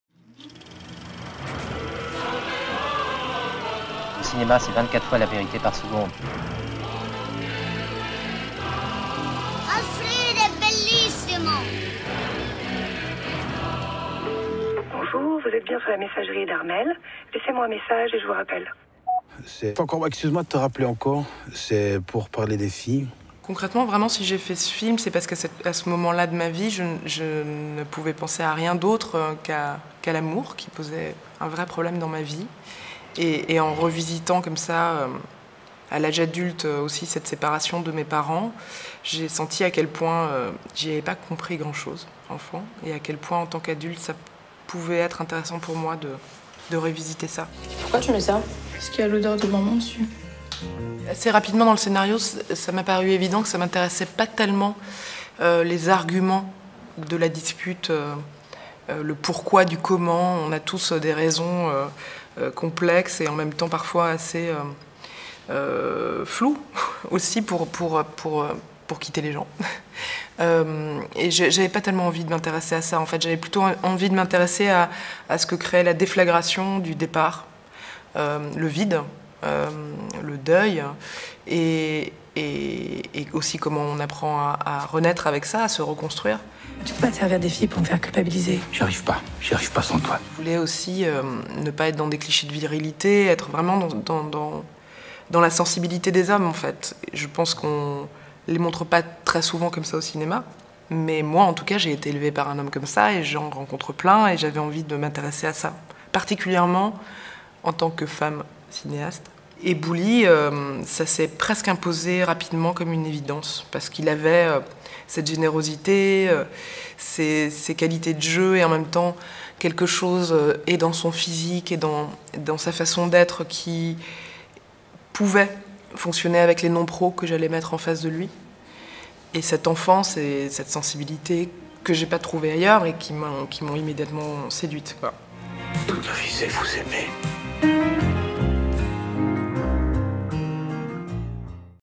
“Indépendance(s) et Création" 2018 : C'est ça l'amour (version courte)
Interview de Claire BURGER à l'occasion de la projection en avant-première de leur film "C'est ça l'amour", lors du festival Ciné32 "Indépendance(s) et Création" 2018

© Ciné 32 / Parlem TV